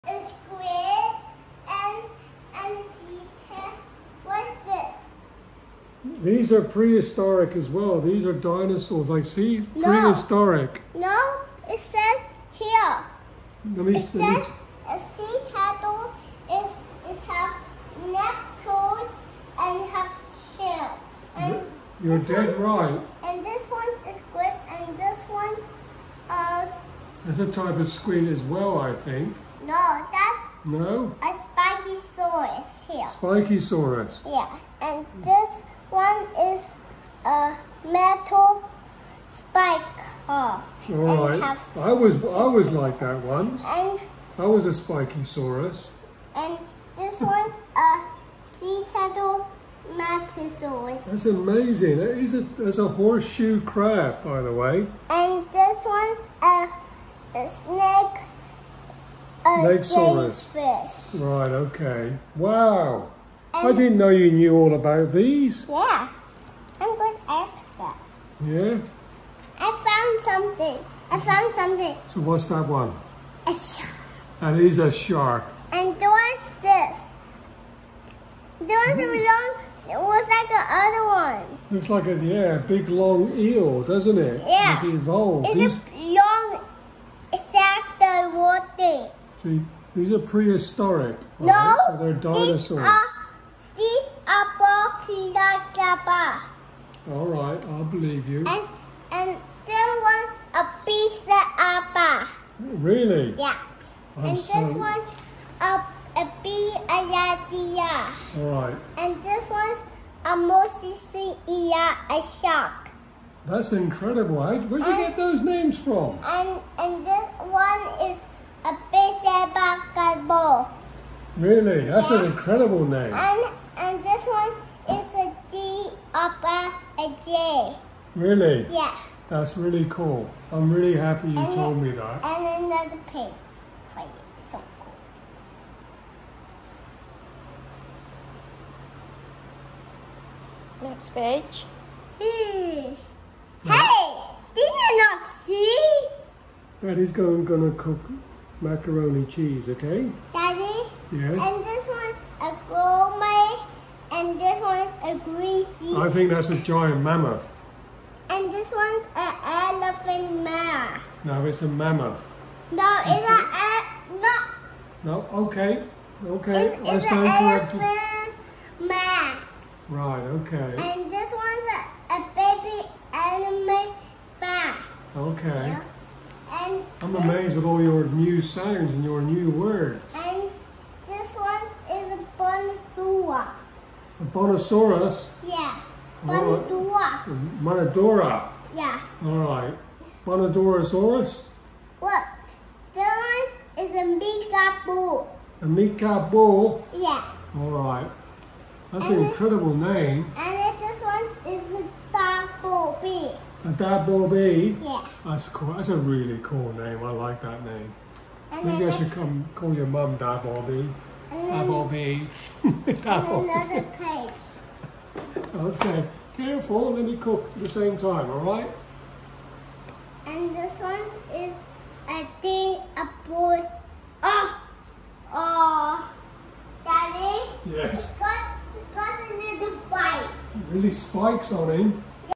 Terlucu, Kenzo ajarin daddy nya hewan Purba
Kenzo ngajarin daddy nya hewan Purba